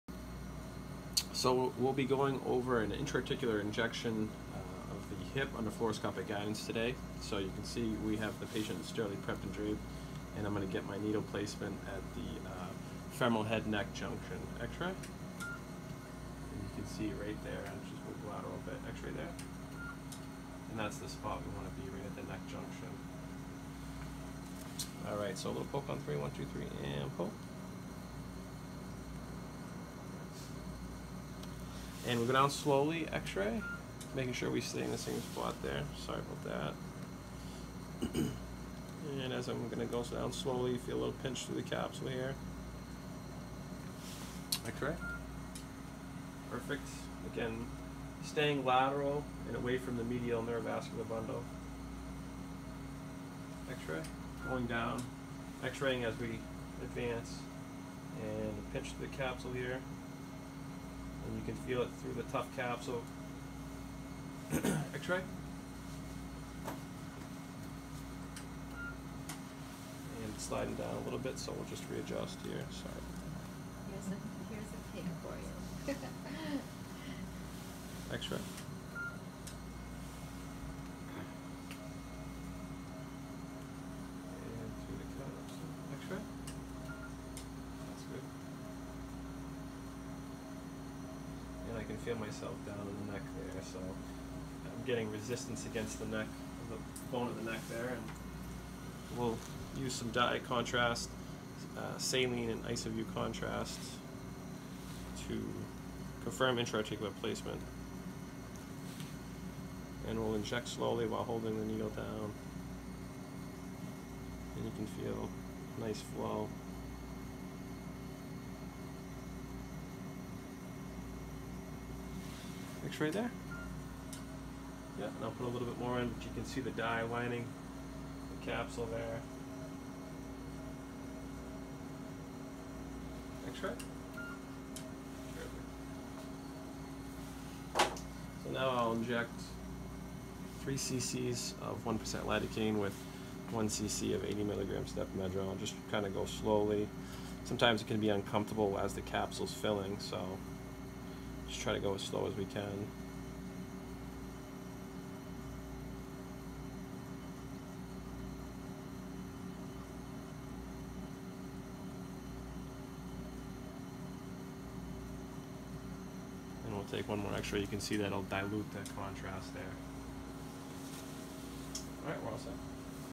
Intra-Articular Injection of the Hip Under Fluoroscopic Guidance
Instructional video on how to perform an intra-articular injection of the hip under fluoroscopic guidance.
Journal: JBJS JOPA
Subspecialty: Hip